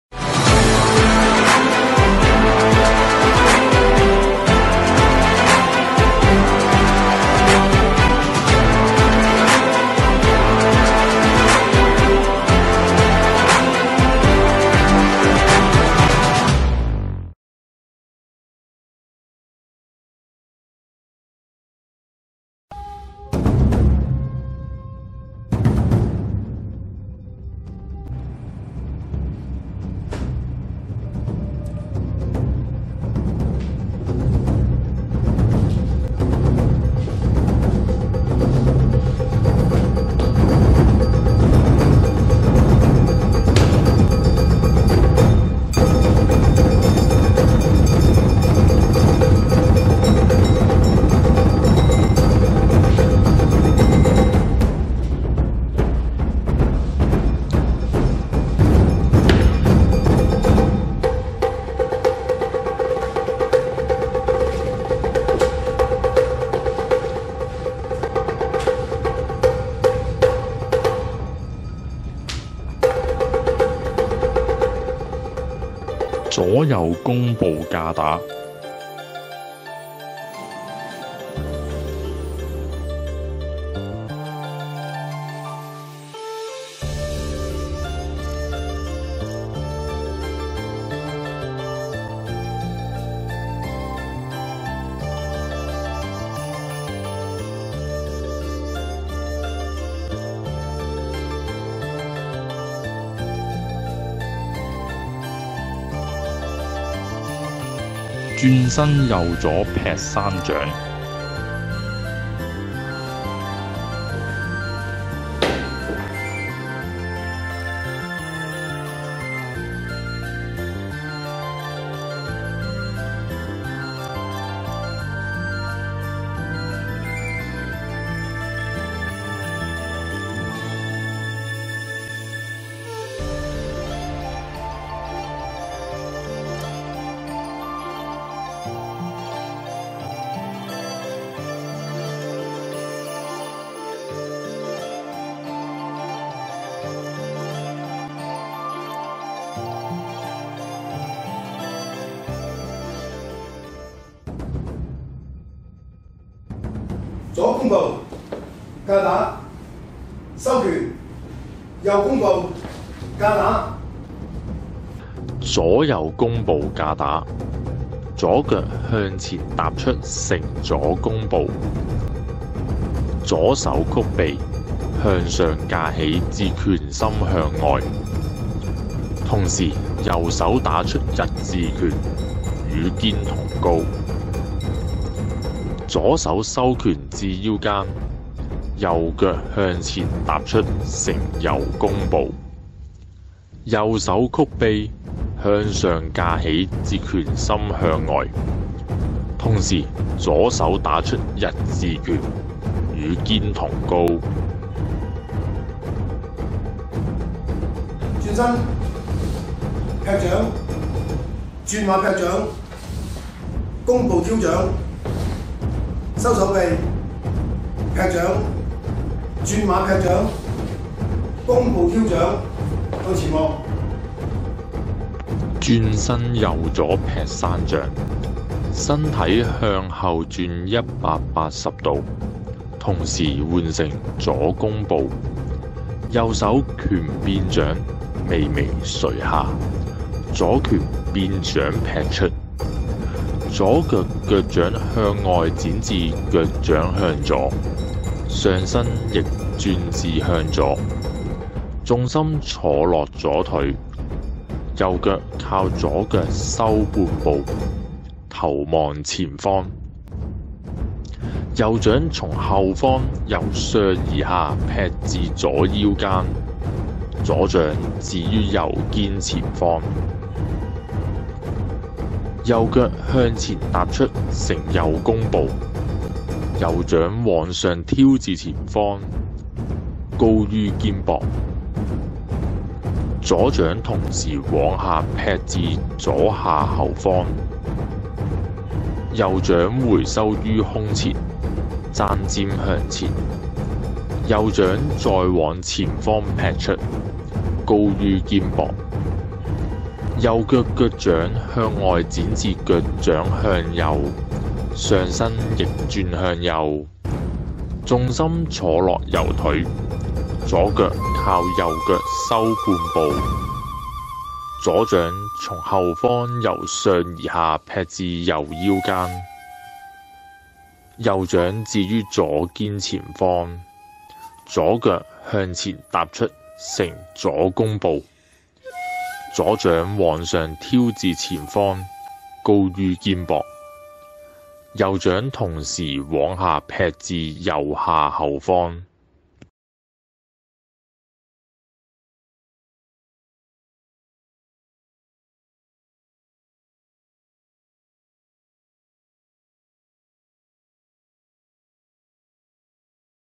大聖劈掛門 -- 基礎篇 -- 第五集（中）
每集將分為（上）（中）（下）三個單元播放，8 集合共 24 個單元；每個星期播出一集，逢週一播（上）、週三播（中）、週五播（下）、週六播放整集內容。
單元（上）：主要是介紹大聖劈掛門的源流歷史、武術知識；
單元（中）：招式示範和招式口訣；
單元（下）：招式動作標準的詳細解說，另加「健身小貼士」。